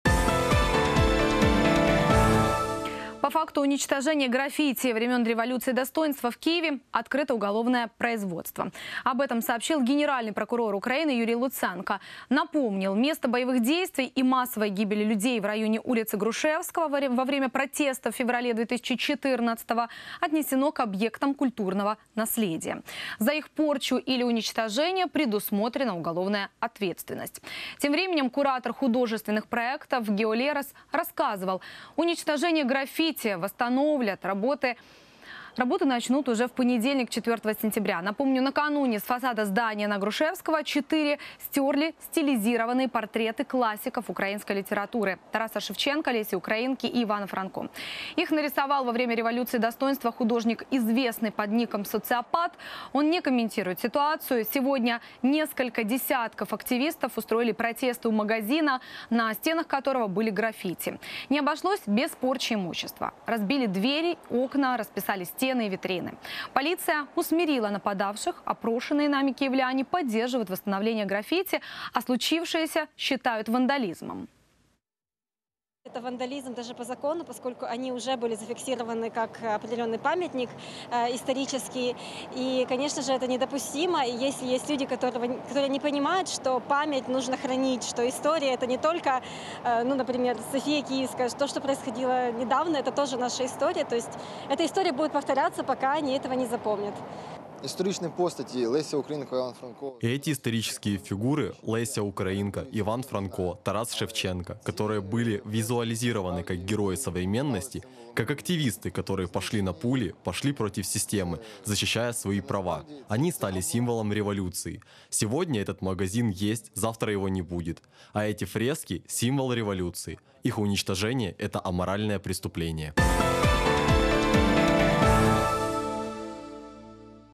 Граффити восстановят
По факту уничтожения граффити времен Революции достоинства в Киеве - открыто уголовное производство. Об этом сообщил Генеральный прокурор Украины Юрий Луценко. Напомнил: место боевых действий и массовой гибели людей в районе улицы Грушевского во время протестов в феврале 2014 отнесено к объектам культурного наследия. За их порчу или уничтожение - предусмотрена уголовная ответственность. Тем временем куратор художественных проектов Гео Лерос рассказал: уничтоженные граффити - восстановят, работы начнут уже в понедельник, 4 сентября. Напомню, накануне с фасада здания на Грушевского, 4, стерли стилизированные портреты классиков украинской литературы - Тараса Шевченко, Леси Украинки и Ивана Франко. Их нарисовал во время Революции достоинства художник известный под ником Социопат. Он не комментирует ситуацию. Сегодня несколько десятков активистов устроили протесты у магазина, на стенах которого были граффити. Не обошлось без порчи имущества: разбили двери и окна, расписали стены, витрины. Полиция усмирила нападавших. Опрошеные нами киевляне поддерживают востановление граффити, а случившиеся считают вандализмом.